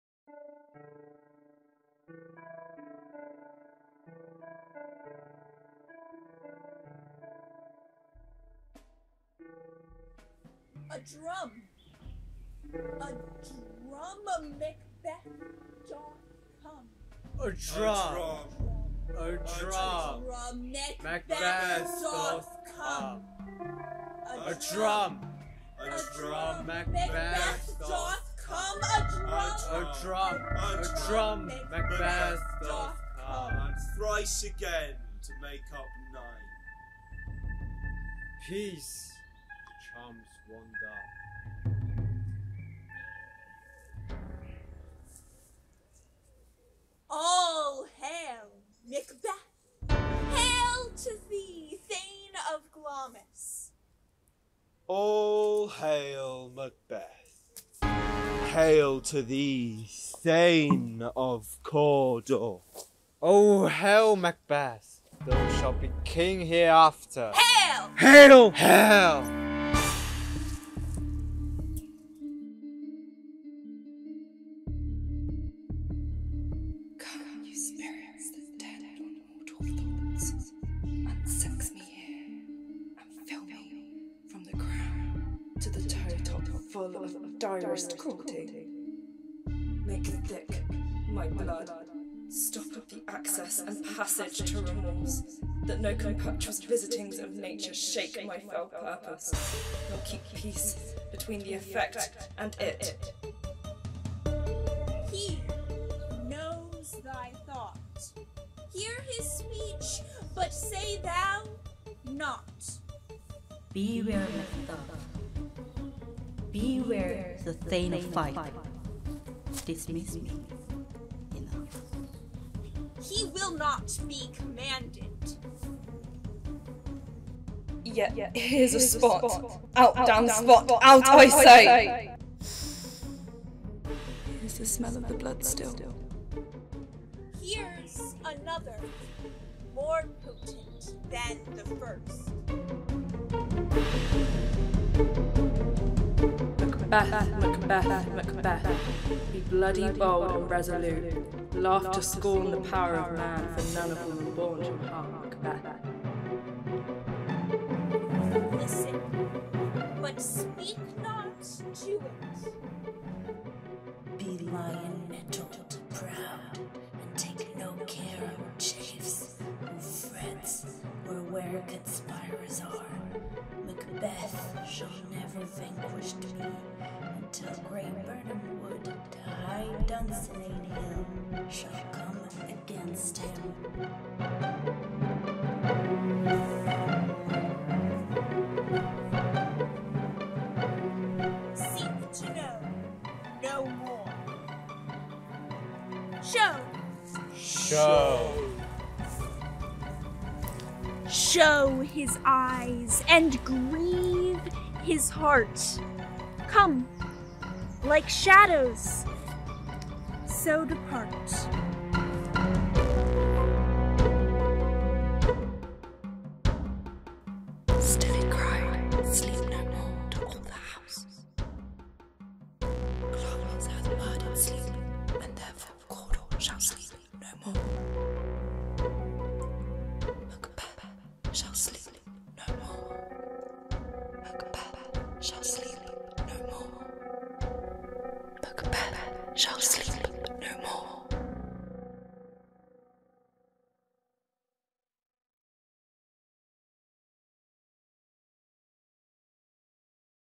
Macbeth - a short film in VR
The VR film is part of an immersive and VR production adapted from parts of Macbeth, called "Can the Devil Speak True?" led by Will Shüler - PDA Director of UG Education at Royal Holloway. The performance research question is: how can immersive theatre and VR tell stories together? We are using prototype new Oculus technology with the support of StoryFutures.

The production is a collaboration from across the School of Performing and Digital Arts:

Film Directed by Tatiana Collet Apraxine and Juno Min
Music composed by Jordan Lewis and Jasmine Bates.
Costume design by Zosia Hao
Witches: Aaron Barrow, Andy Yan, Anna Seyerle
Lady Macbeth: Manuela Perez Gomez
Apparitions: Manuela Perez Gomez, Juno Min, Zosia Hao

Banquo (Live performance): Sean Lin
Ross (Live performance): Jake Stevens

Many thanks for the support from Peggy Ainsworth (Art Collections) and Andy Woods (Media Arts).